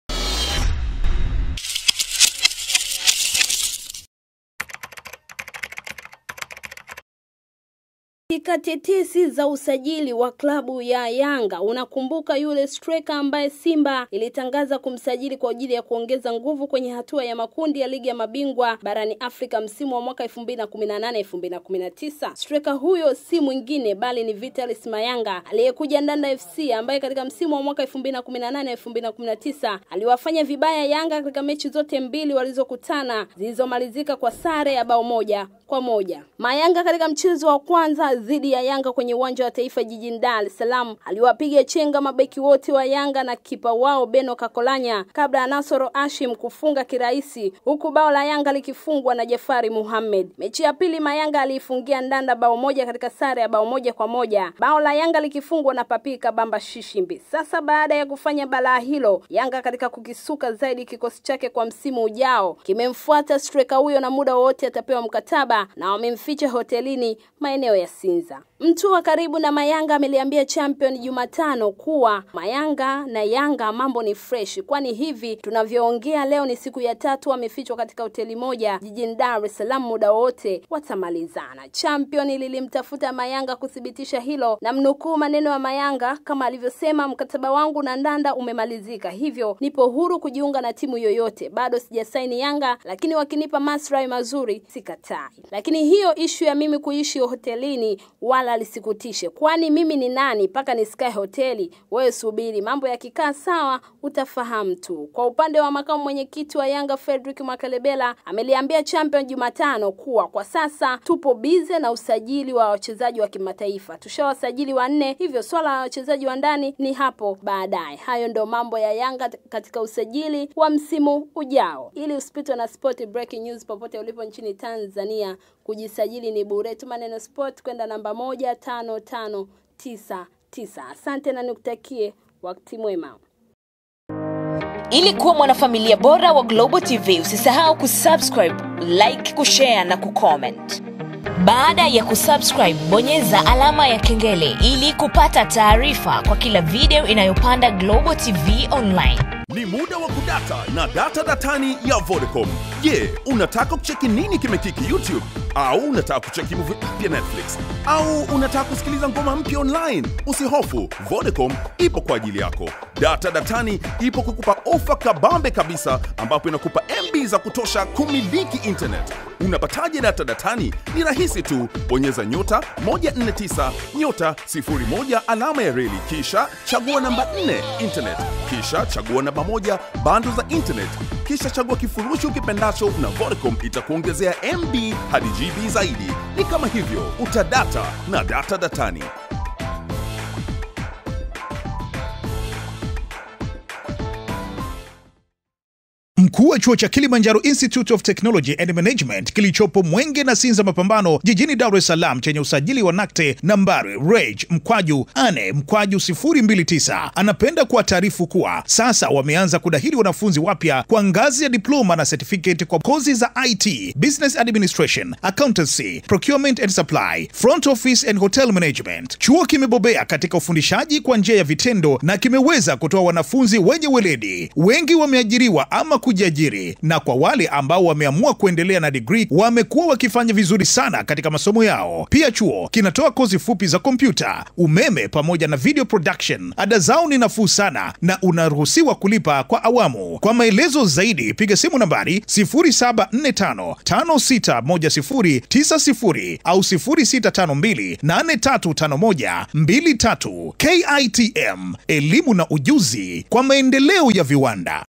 YANGA Yaficha STRAIKA SIMBA Hotelini: USAJILI YANGA
USAJILI YANGA: YANGA YAMFICHA HOTELINI STRAIKA SIMBA

UNAMKUMBUKA yule Starika ambaye Simba ilitangaza kumsajili kwaajili ya kuongeza nguvu kwenye hatua ya makundi ya Ligi ya Mabingwa Barani Afrika msimu wa 2018/2019

Straika huyo si mwingine bali ni vitalis Mayanga aliyekuwa Ndanda FC ambaye katika msimu wa 2018/2019 aliwafanya vibaya Yanga katika mechi zote mbili walizokutana zilizomalizika kwa sare ya 1-1.

FaceBook Instagram Twitter Install Global App: ANDROID